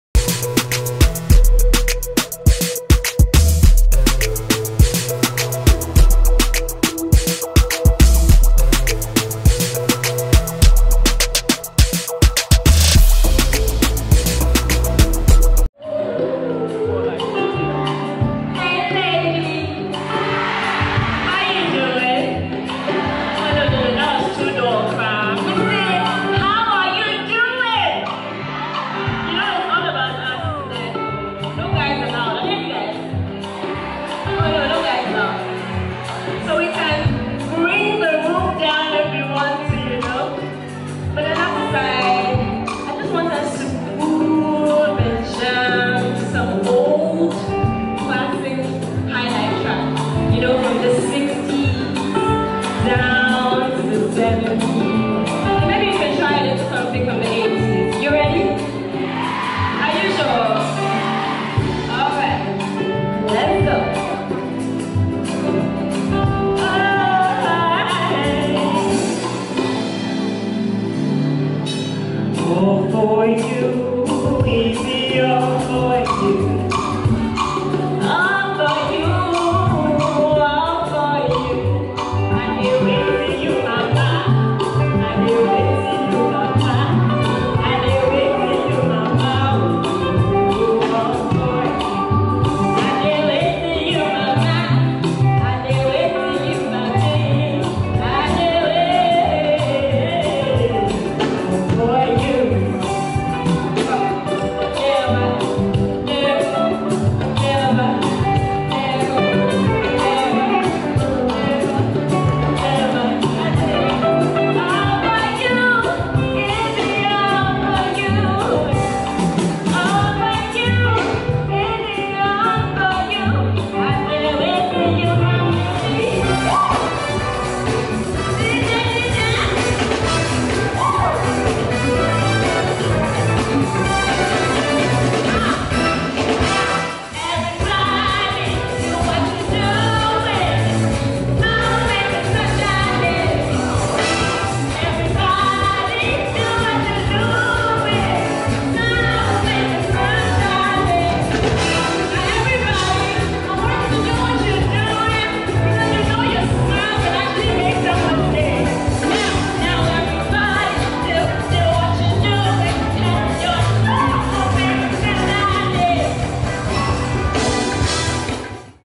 Adomaa - Performance at Girl Talk concert 2015 with Efya | GhanaMusic.com Video
Adomaa performance at Girl Talk concert 2015 with Efya (C) 2015. Palm Media